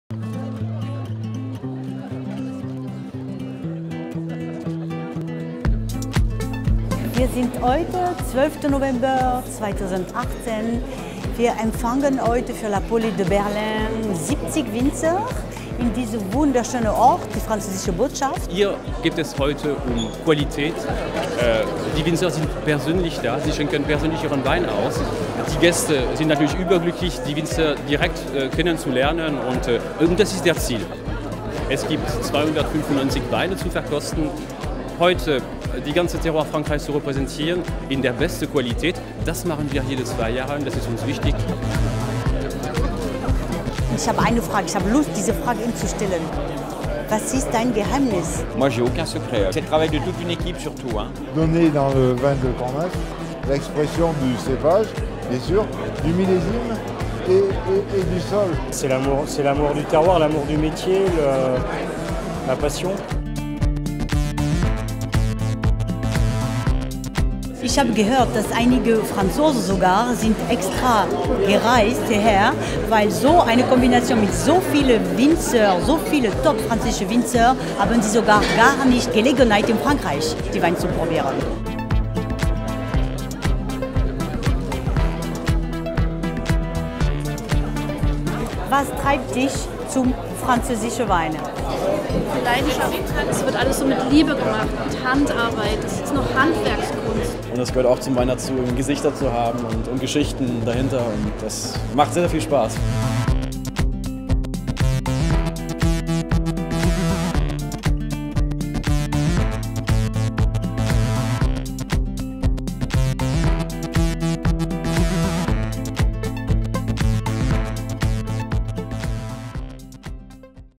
La Paulée de Berlin - BANDE ANNONCE - Edition 2018
LA PAULÉE de BERLIN ist eine exklusive Premiumveranstaltung, die alle zwei Jahre in Berlin stattfindet!

Präsentation
Über 60 Winzer aus unserem Programm aus ganz Frankreich präsentieren persönlich ihre Weine in den Salons der Französischen Botschaft. Unter ihnen finden Sie etablierte Persönlichkeiten, die weltweit Kultstatus besitzen, sowie aufstrebende junge Talente und echte Geheimtipps. Diese Winzer sind charakterstarke Produzenten, die handwerklich erzeugte Terroirweine anbieten.
Schlendern Sie durch die Salons, entdecken und verkosten Sie Ihre Favoriten aus ganz Frankreich, und erfahren Sie direkt von den Winzern alles über Weine, Terroir und die individuelle Arbeit im Keller. Hier treffen Sie auf Sommeliers aus der gehobenen Gastronomie sowie Einkäufer aus dem Weinhandel.
Anmeldungen über unseren Ticketshop beginnen erst am 1. September vor der Veranstaltung.

Gala-Diner
Nach der Präsentation möchten wir die französische Weinkultur und Tradition in Form einer Paulée* mit einem Gala-Diner im Restaurant Grill Royal Berlin zelebrieren. In feierlicher Atmosphäre teilen die Winzer ihre besten Weine an diesem einzigartigen Abendessen und bringen dafür ganz besondere Weine und Jahrgänge mit, oft in Magnumflaschen oder noch größeren Formaten.
Auch Sie sind herzlich eingeladen, besondere Flaschen aus Ihren eigenen Reserven mitzubringen, um diese mit den Winzern und Gästen zu teilen. Reservierungen nehmen wir gerne erst ab dem 1. September vor der Veranstaltung entgegen.

*Die Paulée, was „Schaufel“ bedeutet und vom burgundischen Dialektwort paule („Schaufel“) stammt, bezeichnet traditionell das Mahl, das die Weinlese in Burgund abschließt. Dieser Begriff symbolisierte lange Zeit die Feier des neuen Weins, gewann jedoch ab Ende des 19. Jahrhunderts eine festlichere Bedeutung und bezieht sich nun auf das Fest, das nach der Auktion der Hospices de Beaune stattfindet. Heutzutage ist die Paulée de Meursault eines der meist erwarteten Weinevents, bei dem Winzer und Weinliebhaber zusammenkommen, um das Ende der Weinlese zu feiern.